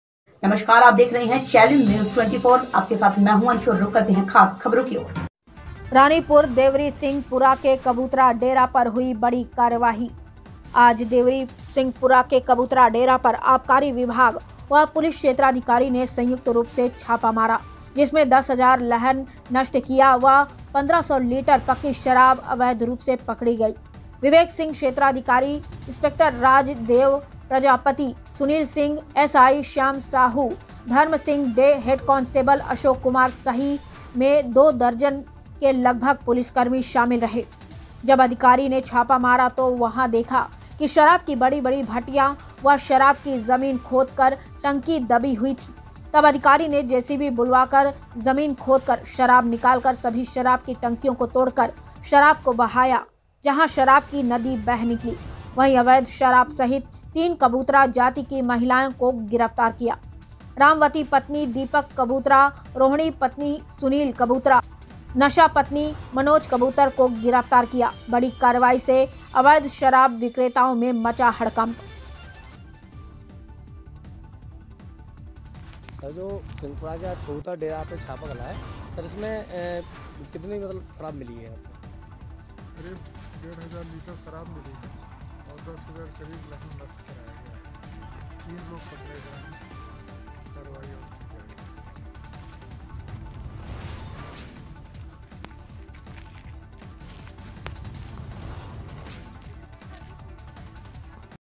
रानीपुर देवरीसिंह पुरा के कबूतरा डेरा पर हुई बड़ी कार्यवाही
,,,,,,,,,,,,,,,,,,,,,,,,,,, आज देवरी सिंह पुरा के कबूतरा डेरा पर आबकारी विभाग बा पुलिस क्षेत्राधिकारी ने संयुक्त रूप से मारा छापा जिसमें 10,000 लहर नष्ट किया बा 1500 लीटर पक्की शराब अवैध रूप से पकड़ी गई विवेक सिंह क्षेत्राधिकारी  स्पेक्टर राज देव प्रजापति सुनील सिंह एसआई श्याम साहू धर्म सिंह हेड कांस्टेबल अशोक कुमार सही में 2 दर्जन के लगभग पुलिसकर्मी शामिल रहे जबअधिकारी ने छापा मारा तो वहां देखा शराब की बड़ी-बड़ी भटियां बा शराब की जमीन खोदकर टंकी दबी हुई थी तब अधिकारी ने जेसीबी बुलवाकर जमीन खोदकर शराब निकालकर सभी शराब की टंकियों को तोड़कर शराब को बहाया जहां शराब की नदी बह निकली वही अवैध शराब सहित 3 कबूतरा जाति की महिलाएं को गिरफ्तार किया रामवती पत्नी दीपक कबूतरा रोहिणी पत्नी सुनील कबूतर नशा पत्नी मनोज कबूतर को गिरफ्तार किया बड़ी कार्रवाई से अवैध शराब विक्रेताओं में मचा हड़कंप